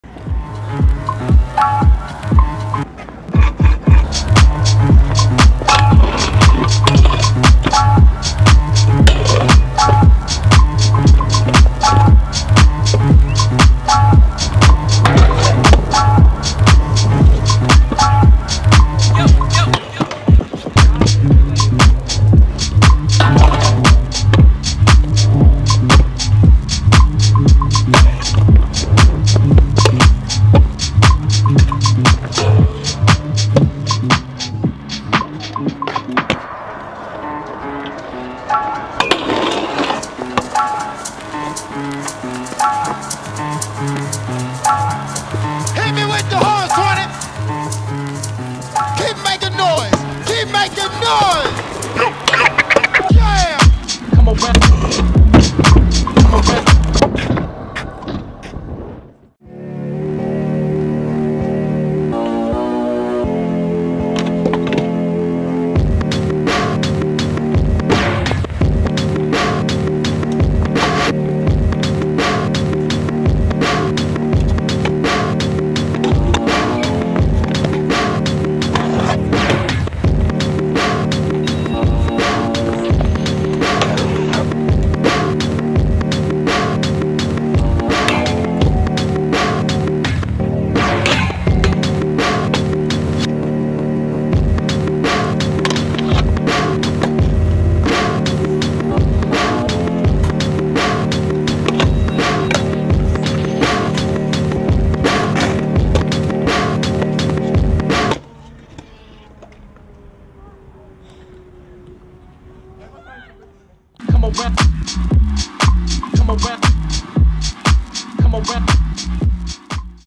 Артём Щербаков - Summer Leftovers
У ребят из нашей команды скопилось немало материала за прошлый сезон, Артём Щербаков - не исключение! Представляем вам "Summer Leftovers" с его участием - коллекцию клипов, отснятую летом 22-ого в московских скейтпарках.

Съёмка: Тимофей Соседкин (@drecyj)
Монтаж: Костя Киктенко (@kiktik)